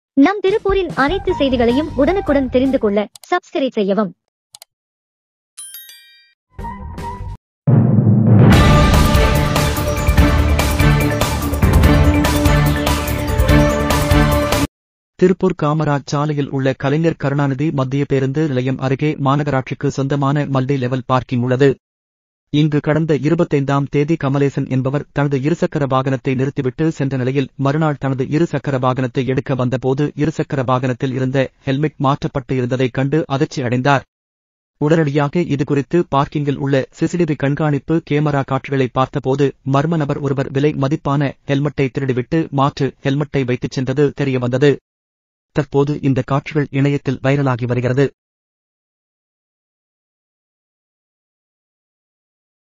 உஷார் மக்களே ! ஹெல்மெட் திருட்டு ! வைரல் சிசிடிவி
திருப்பூர் காமராஜ் சாலையில் உள்ள கலைஞர் கருணாநிதி மத்திய பேருந்து நிலையம் அருகே மாநகராட்சிக்கு சொந்தமான மல்டி லெவல் பார்க்கிங் உள்ளது.   இங்கு கடந்த 25ஆம் தேதி கமலேசன் என்பவர் தனது இருசக்கர வாகனத்தை நிறுத்திவிட்டு சென்ற நிலையில் மறுநாள் தனது இரு சக்கர வாகனத்தை எடுக்க  வந்தபோது இருசக்கர வாகனத்தில் இருந்த ஹெல்மெட் மாற்றப்பட்டு இருந்ததை கண்டு அதிர்ச்சி அடைந்தார். உடனடியாக இது குறித்து பார்க்கிங்கில் உள்ள சிசிடிவி கண்காணிப்பு கேமரா காட்சிகளை பார்த்தபோது மர்ம நபர் ஒருவர் விலை மதிப்பான ஹெல்மட்டை திருடிவிட்டு மாற்று  ஹெல்மட்டை வைத்துச் சென்றது தெரியவந்தது.  தற்போது இந்த காட்சிகள் இணையத்தில் வைரலாகி வருகிறது.